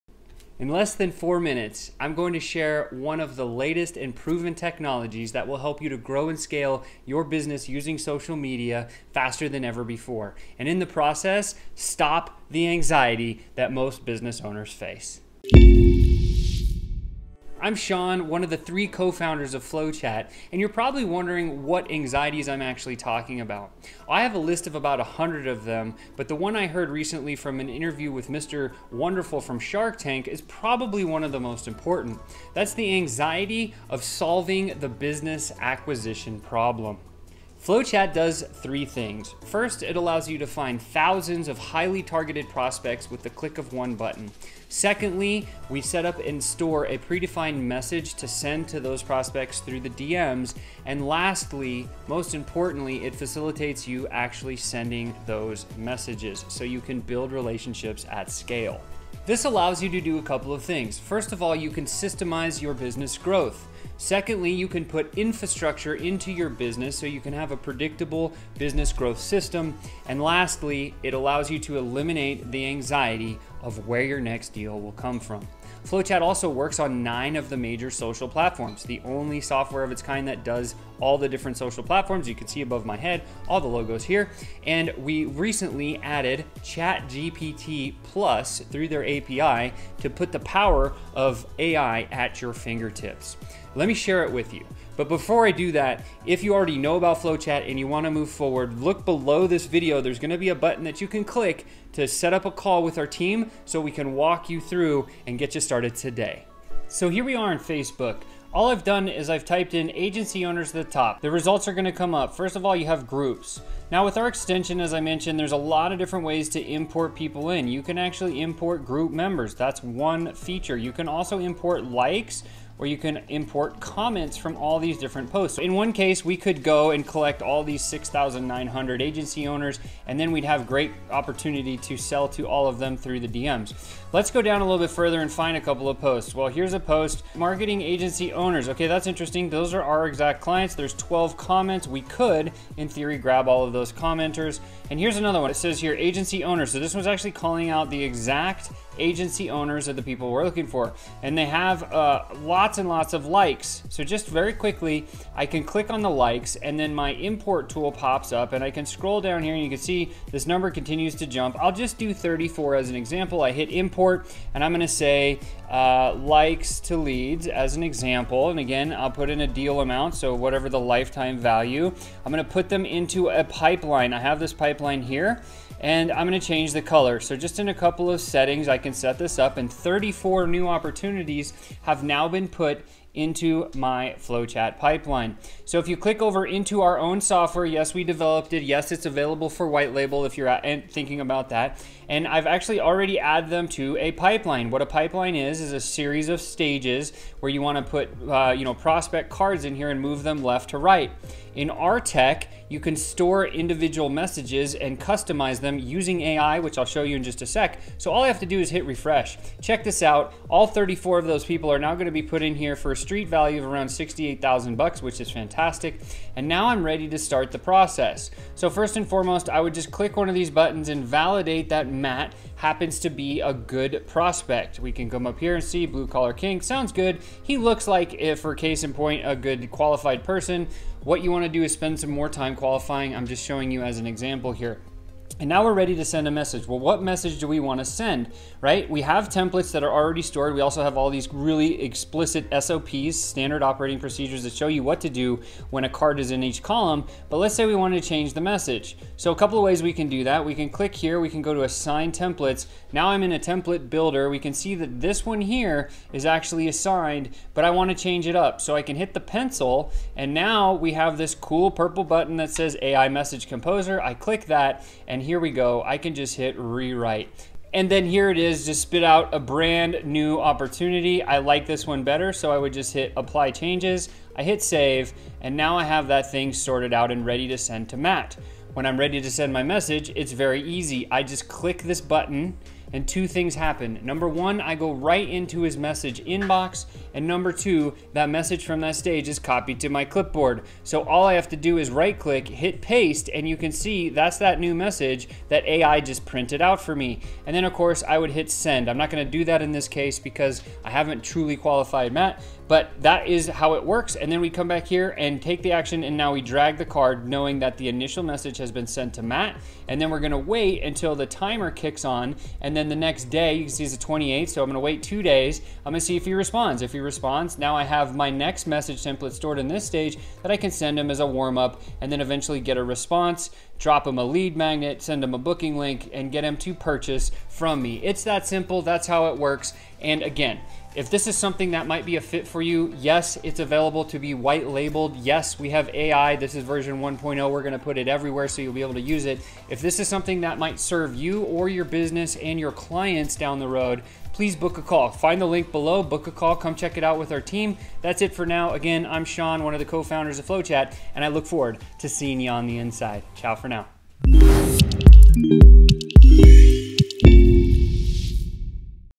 FlowChat: Breakthrough Technology At Your Fingertips Overview v4.0
Effortless Sales Prospecting with FlowChat: Boost Your Business on Social Media!

Introducing FlowChat, your ultimate social selling companion. Unlike Flowcharts, which help you plan your business strategies, FlowChat is a powerful software designed to manage all your social media sales prospecting in one central location, enabling you to create an extensive pipeline of highly-targeted leads for your business.

Regardless of the size of your social media audience, FlowChat can enhance your sales prospecting efforts without the need for additional ad expenditure. Experience the unparalleled benefits of the #1 Ultimate Social Selling Machine and elevate your business with FlowChat.

Developed by sales professionals with your needs in mind, FlowChat is specifically tailored for small to medium-sized businesses with one or more salespeople on their team.

More specifically, we help you to white-label the software, create your own monthly recurring revenue (MRR), and offer a unique software-as-a-service solution to your clients.

This innovative sales pipeline message management software is perfect for those seeking to expand their online presence. Simplify your entire sales prospecting process with just a few clicks, as FlowChat seamlessly integrates with Go High Level, Salesforce, and numerous other CRM tools, providing sales pipeline management that scales with your business.

Unlock the potential to close more deals with FlowChat.

Happy selling!

sean